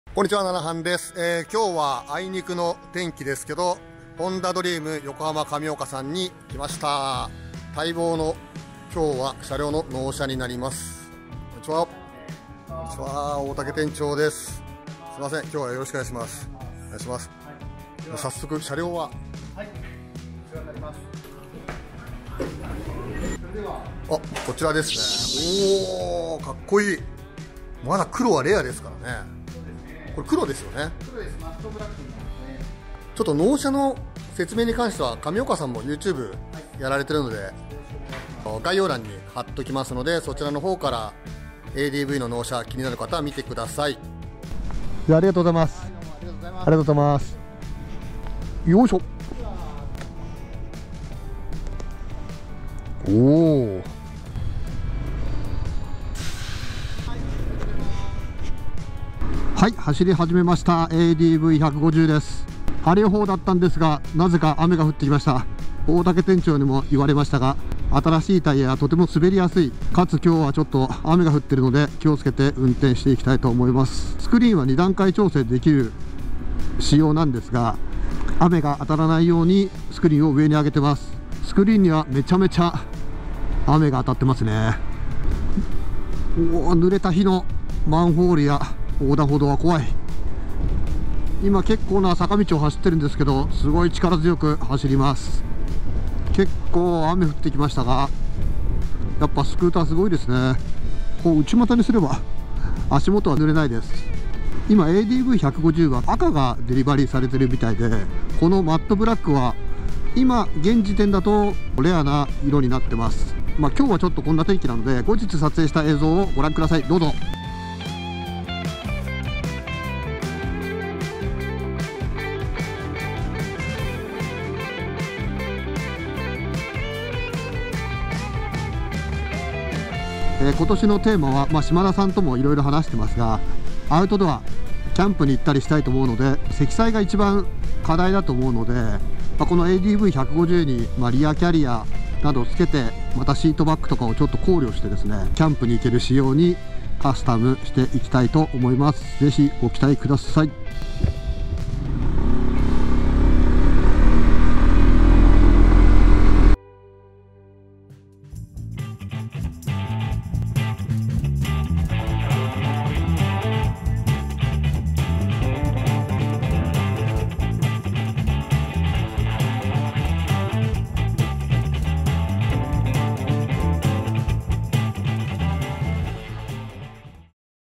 ホンダ ADV150 マットガンパウダーメタリックを納車！雨天走行しながら次の展開を考えます！/ Motorcycle Fantasy
色々な方の協力を受けながら、
サブスクリプションのような形で納車していきます♪

▼撮影機材
DJI Osmo Pocket  1080/60